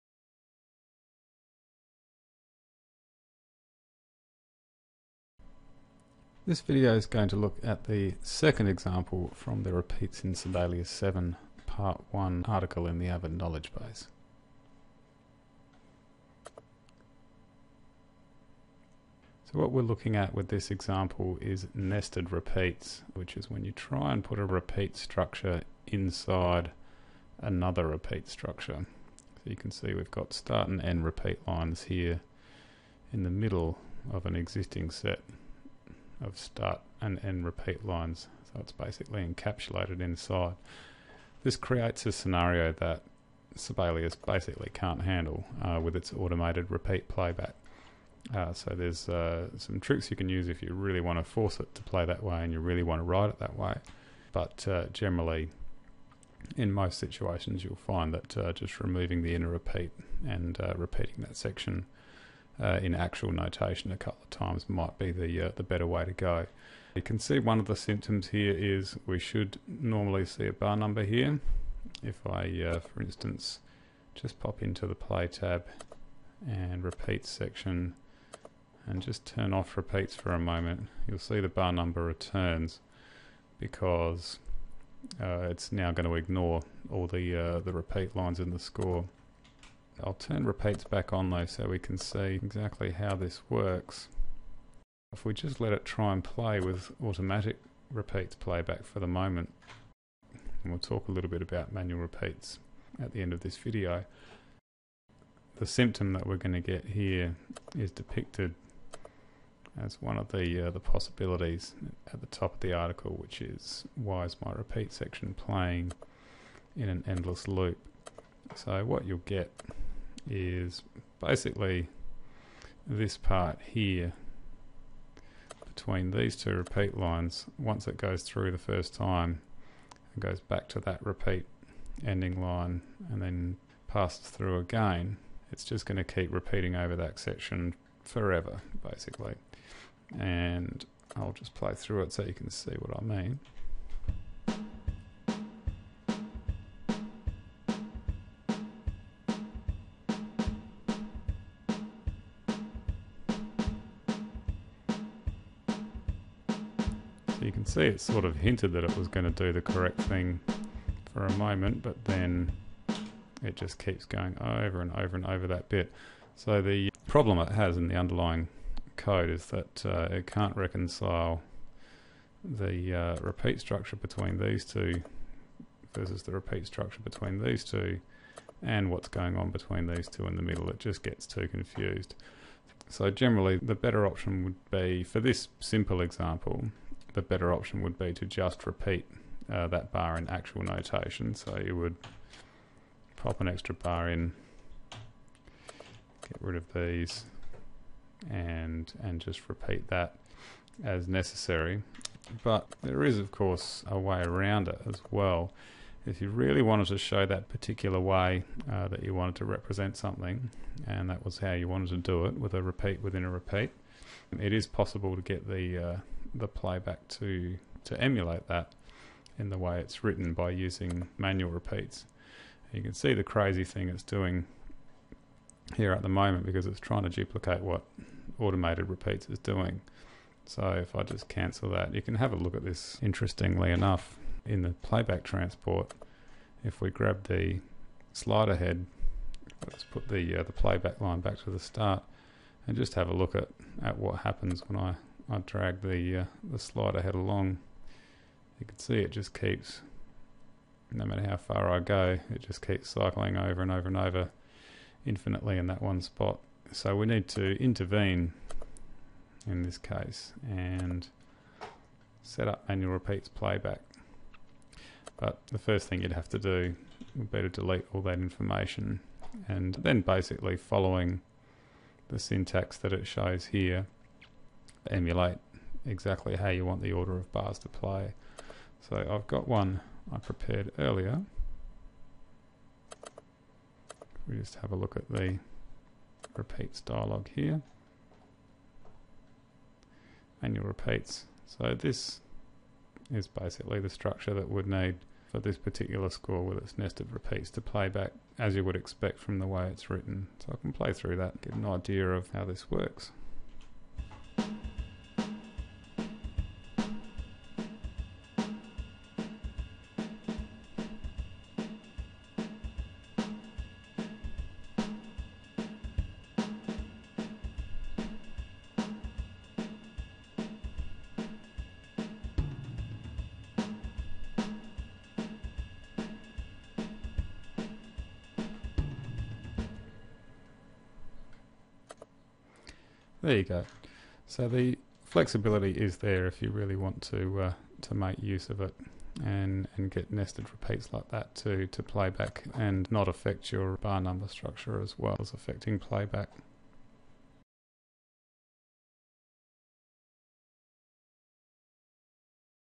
Avid Support: How Avid Sibelius 7 Repeat Structure Can Be Broken (Example 2)
Repeat structures in Avid Sibelius 7 generally work without issue, however problems may present when using repeats, if they're not entered in a way that Sibelius can automatically handle them.

Know what causes these issues and what you can do to avoid them by following the guidelines in this Avid Knowledge Base article.